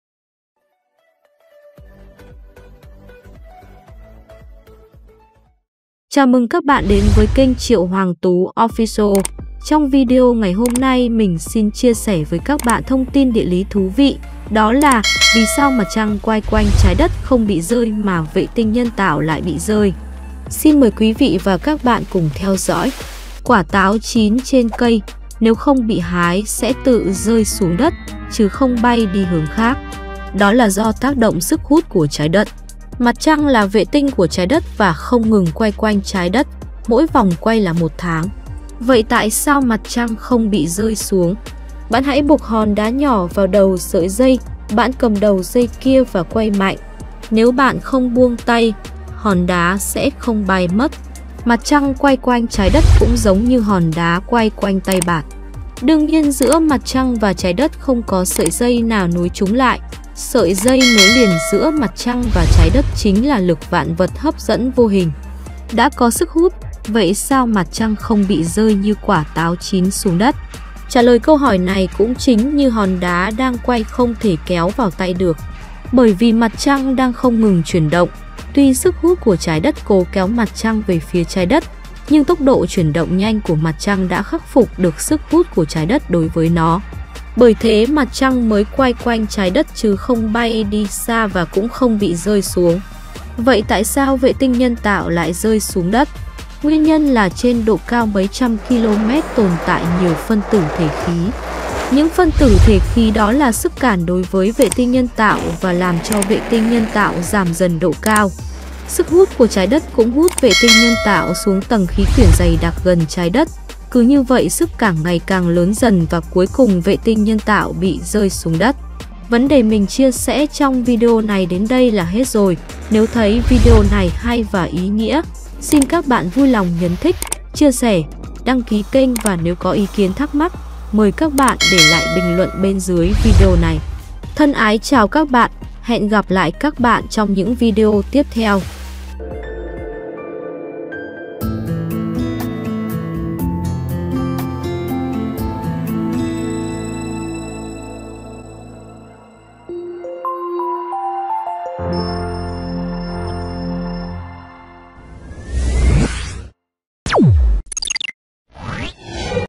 Vì sao mặt trăng quay quanh trái đất không bị rơi mà vệ tinh nhân tạo lại bị rơi?
✅ Nội dung chính của Video: Vì sao mặt trăng quay quanh trái đất không bị rơi mà vệ tinh nhân tạo lại bị rơi? Quả Táo chín trên cây, Nếu không bị hái sẽ tự rơi xuống đất, chứ không bay đi hướng khác. đó là do tác động sức hút của trái đất.
Mặt trăng là vệ tinh của trái đất và không ngừng quay quanh trái đất, mỗi vòng quay là một tháng. Vậy Tại sao mặt trăng không bị rơi xuống?
 Bạn hãy buộc hòn đá nhỏ vào đầu sợi dây, bạn cầm đầu dây kia và quay mạnh. Nếu bạn không buông tay, Hòn Đá sẽ không bay mất. mặt trăng quay quanh trái đất cũng giống như hòn đá quay quanh tay bạn. đương nhiên giữa mặt trăng và trái đất không có sợi dây nào nối chúng lại, sợi dây nối liền giữa mặt trăng và trái đất chính là lực vạn vật hấp dẫn Vô Hình.
 Đã có sức hút, vậy sao mặt trăng không bị rơi như quả táo chín xuống đất? trả lời câu hỏi này cũng chính như hòn đá đang quay không thể kéo vào tay được. bởi vì Mặt Trăng đang không ngừng chuyển động, Tuy sức hút của trái đất Cố kéo mặt trăng về phía trái đất, nhưng tốc độ chuyển động nhanh của mặt trăng đã khắc phục được sức  hút của trái đất đối với nó. bởi thế Mặt Trăng mới quay quanh trái đất chứ không bay đi xa và cũng không bị rơi xuống.
 vậy tại sao vệ tinh nhân tạo lại rơi xuống đất? Nguyên nhân là trên độ cao mấy trăm ki lô mét tồn tại nhiều phân tử thể khí. những phân tử thể khí đó là sức cản đối với vệ tinh nhân tạo và làm cho vệ tinh nhân tạo giảm dần độ cao. sức hút của trái đất cũng hút vệ tinh nhân tạo xuống tầng khí quyển dày đặc gần trái đất, cứ như vậy sức cảng ngày càng lớn dần và cuối cùng vệ tinh nhân tạo bị rơi xuống đất.